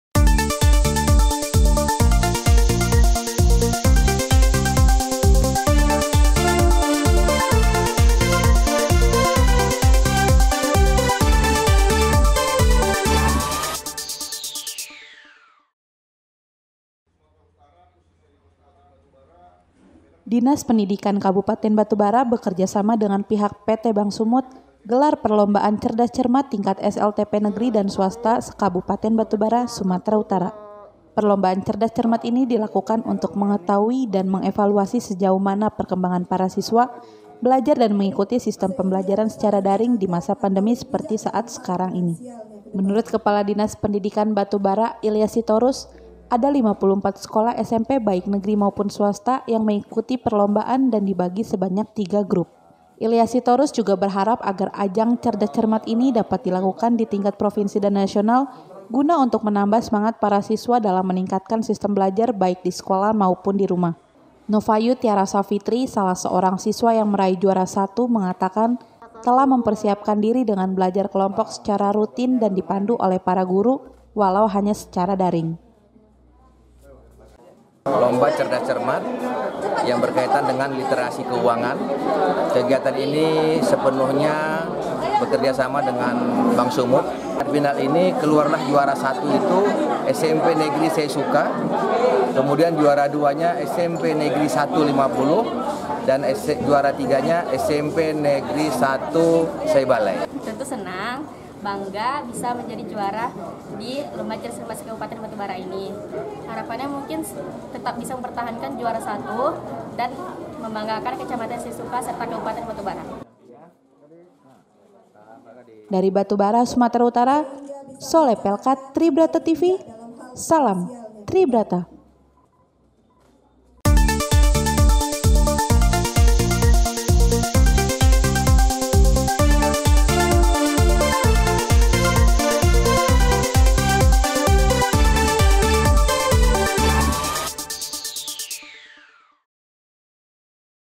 DINAS PENDIDIKAN KABUPATEN BATU BARA GELAR PERLOMBAAN CERDAS CERMAT
Dinas Pendidikan Kabupaten Batu Bara bekerja sama dengan pihak PT. Bank Sumut gelar perlombaan cerdas cermat tingkat SLTP Negeri dan swasta se - kabupaten Batu Bara Sumatera Utara.

Perlombaan cerdas cermat ini dilakukan untuk mengetahui dan mengevaluasi sejauh mana perkembangan para siswa belajar dan mengikuti sistem pembelajaran secara daring di masa pandemi seperti saat ini.
Menurut kepala Dinas Pendidikan Batu Bara Ilyas Sitorus ada 54 sekolah SMP baik negeri maupun swasta yang mengikuti perlombaan dan dibagi sebanyak 3 group.

Ilyas Sitorus juga berharap agar ajang cerdas cermat ini dapat dilakukan di tingkat provinsi dan nasional guna untuk menambah semangat para siswa dalam meningkatkan sistem belajar baik disekolah maupun dirumah.

Novayu Tiara Safitri salah seorang siswa yang meraih juara satu mengatakan, telah mempersiapkan diri dengan belajar kelompok secara rutin dan di pandu oleh para guru walau hanya secara daring.

Salam Tribrata.